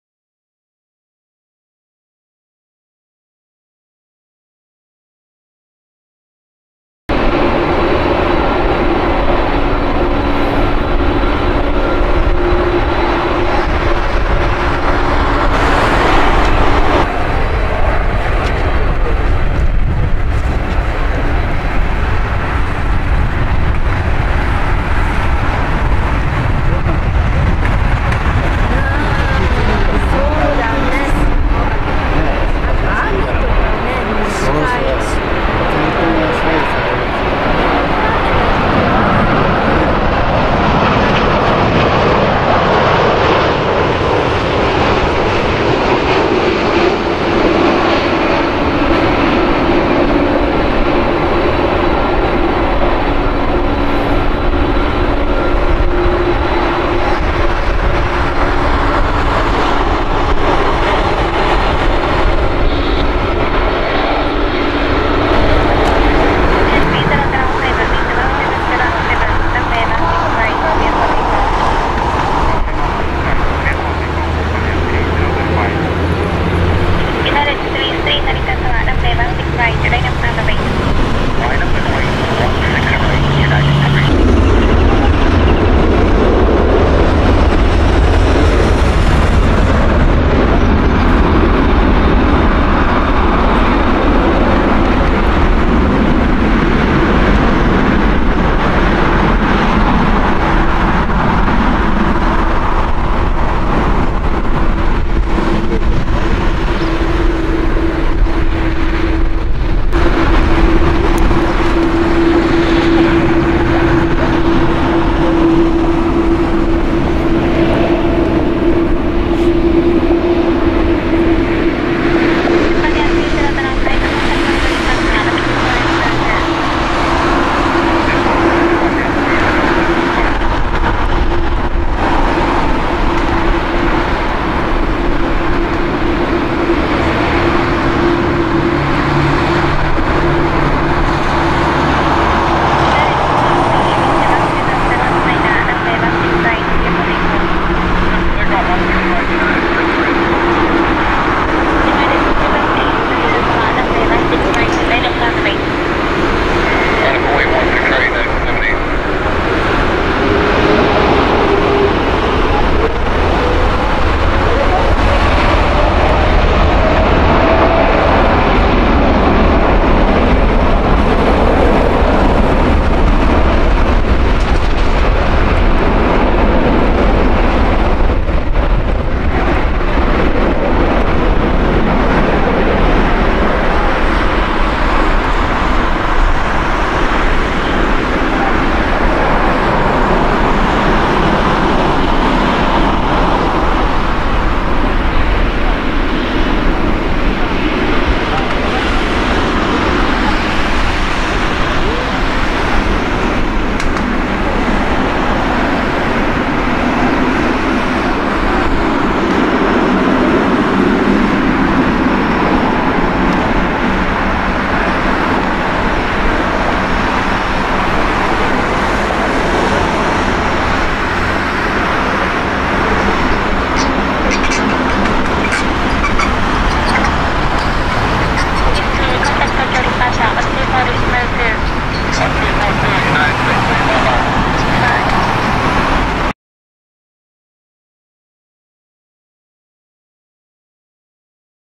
☆ユナイテッド航空☆Boeing 787-9 Dreamliner☆Narita RWY16R成田空港! さくらの山
成田空港　着陸と離陸
ユナイテッド航空 (United Airlines)
Boeing 787-9
N35953